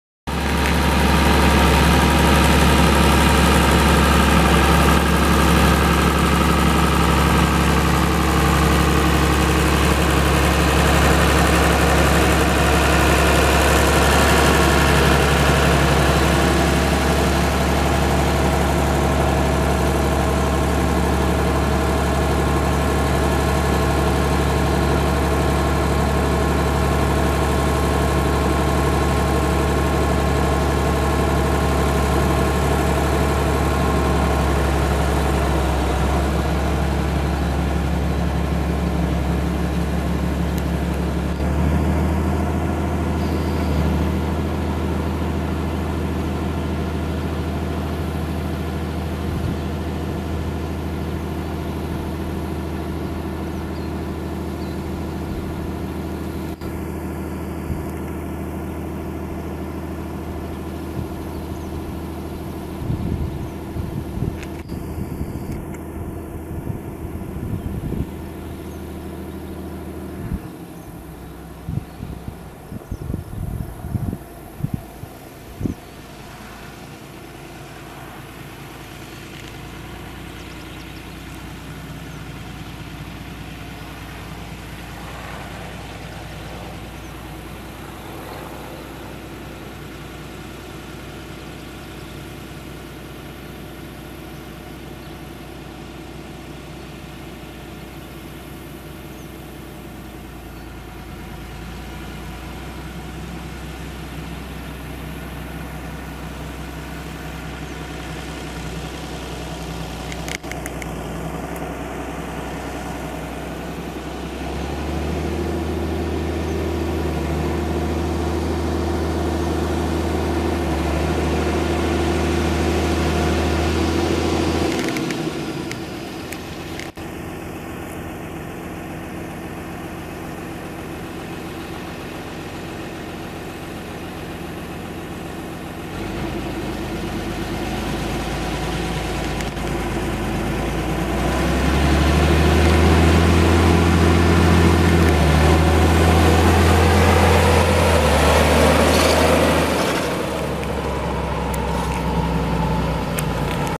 1973 MINNEAPOLIS MOLINE G1355 For Sale
1973 Minneapolis Moline G1355 Tractor - Runs and Operates Great - Clean and Straight - MM D585 Diesel Engine - Hydraulic Shift with Over and Under Transmission - 1000 PTO - 2 Remotes - Cab - Oil Pump is External of Engine (See Pics) Nice Cab and Tires - Pin Hitch - 11-16 Fronts - 18.4-38 Rears - Check Out the Video - Many Collector Tractors Selling at this Auction, Including Oliver, Minneapolis Moline, Cockshutt, Ford and More - Inspections are Welcome - Call to Schedule an Inspection